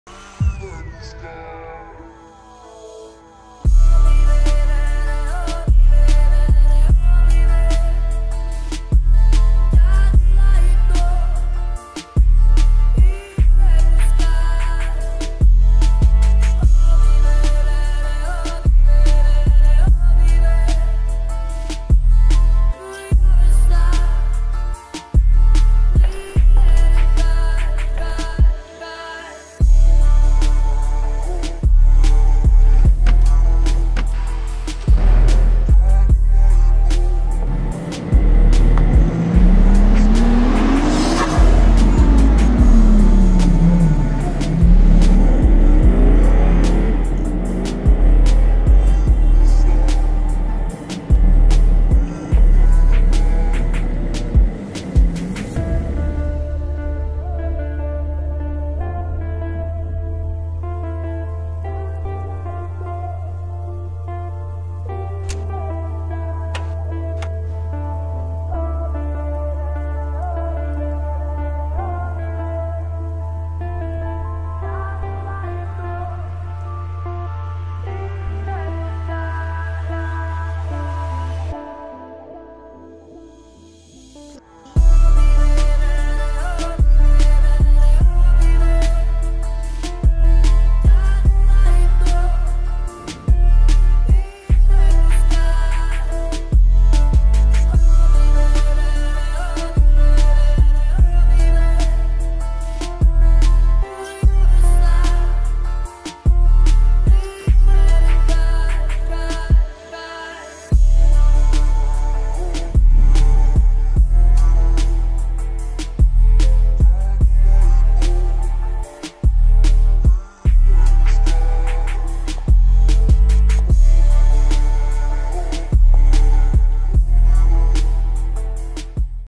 Driving the 1 of 1 Hermès Pagani.
A couple that drives together, stays together!? We think so! So not many people know this but Manny and I actually met on Facebook and we always joke that it was love at first poke! LOL We have a lot of things in common but the first and main thing I would say was our shared love of cars! To this day, the love is still there. This car-if you can even call it that, is EXTRA special to us because it is truly a marriage of two legendary brands: PAGANI HUAYRA & HERMÈS coming together. Manny flew out the entire design team of Hermès Italy to create this 1 of 1 masterpiece.  The rich chocolate carbon fiber exterior perfectly compliments the smooth interior made of classic Hermès leather and the car even comes with a full, bespoke luggage set and yes, my very own Birkin that matches the interior. ♥️